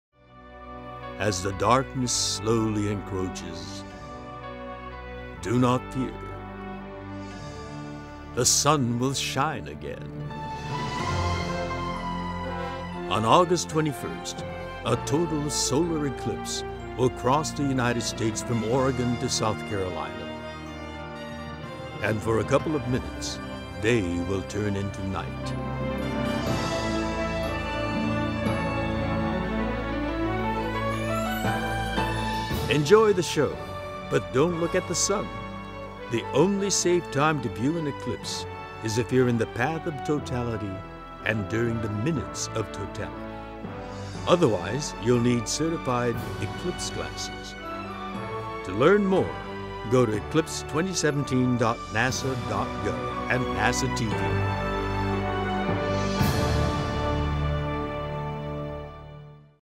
Oh My! George Takei Narrates NASA Video About Solar Eclipse
The Star Trek star and social media phenomenon has some viewing tips for the Total Solar Eclipse of Aug. 21st, 2017.

Credit: NASA